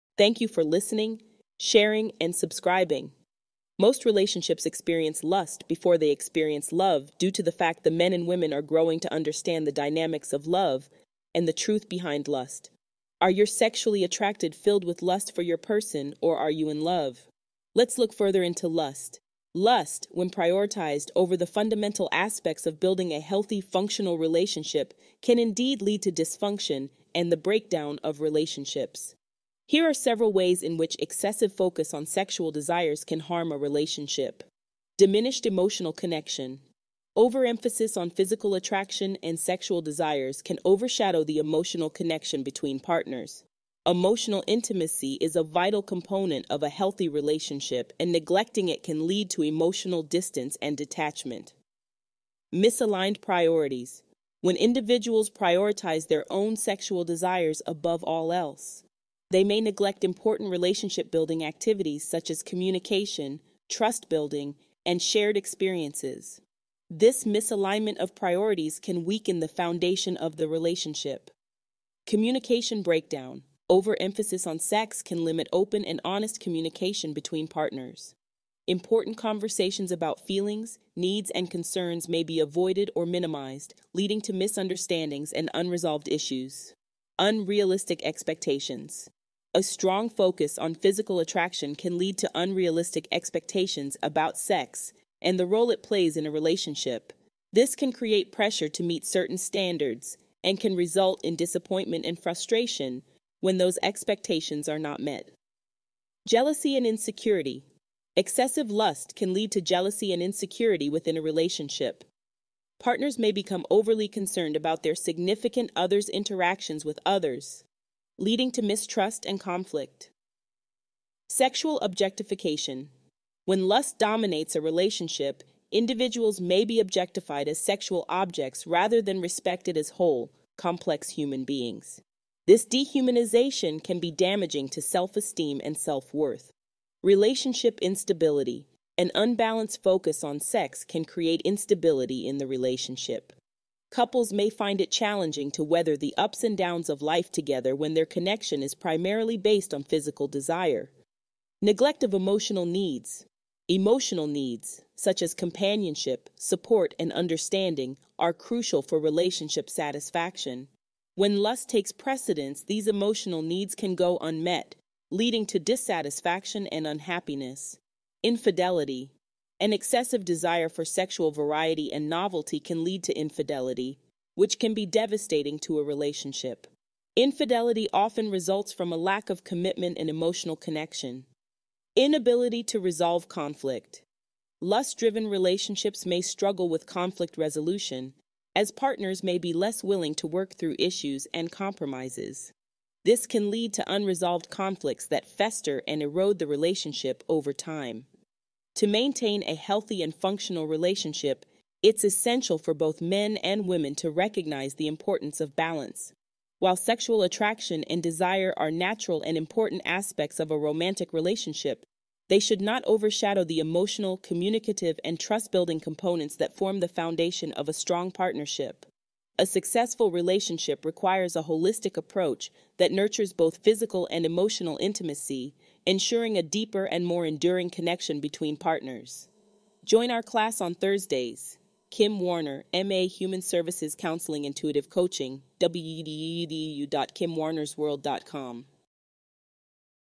Lust or love
Lust, when prioritized over the fundamental aspects of building a healthy, functional relationship, can indeed lead to dysfunction and the breakdown of relationships. Here are several ways in which excessive focus on sexual desires can harm a relationship:

Diminished Emotional Connection: Overemphasis on physical attraction and sexual desires can overshadow the emotional connection between partners. Emotional intimacy is a vital component of a healthy relationship, and neglecting it can lead to emotional distance and detachment.

Misaligned Priorities: When individuals prioritize their own sexual desires above all else, they may neglect important relationship-building activities, such as communication, trust-building, and shared experiences. This misalignment of priorities can weaken the foundation of the relationship.

Communication Breakdown: Overemphasis on sex can limit open and honest communication between partners. Important conversations about feelings, needs, and concerns may be avoided or minimized, leading to misunderstandings and unresolved issues.

Unrealistic Expectations: A strong focus on physical attraction can lead to unrealistic expectations about sex and the role it plays in a relationship. This can create pressure to meet certain standards and can result in disappointment and frustration when those expectations are not met.

Jealousy and Insecurity: Excessive lust can lead to jealousy and insecurity within a relationship. Partners may become overly concerned about their significant other's interactions with others, leading to mistrust and conflict.

Sexual Objectification: When lust dominates a relationship, individuals may be objectified as sexual objects rather than respected as whole, complex human beings. This dehumanization can be damaging to self-esteem and self-worth.

Relationship Instability: An unbalanced focus on sex can create instability in the relationship. Couples may find it challenging to weather the ups and downs of life together when their connection is primarily based on physical desire.

Neglect of Emotional Needs: Emotional needs, such as companionship, support, and understanding, are crucial for relationship satisfaction. When lust takes precedence, these emotional needs can go unmet, leading to dissatisfaction and unhappiness.

Infidelity: An excessive desire for sexual variety and novelty can lead to infidelity, which can be devastating to a relationship. Infidelity often results from a lack of commitment and emotional connection.

Inability to Resolve Conflict: Lust-driven relationships may struggle with conflict resolution, as partners may be less willing to work through issues and compromises. This can lead to unresolved conflicts that fester and erode the relationship over time.

To maintain a healthy and functional relationship, it's essential for both men and women to recognize the importance of balance. While sexual attraction and desire are natural and important aspects of a romantic relationship, they should not overshadow the emotional, communicative, and trust-building components that form the foundation of a strong partnership. A successful relationship requires a holistic approach that nurtures both physical and emotional intimacy, ensuring a deeper and more enduring connection between partners.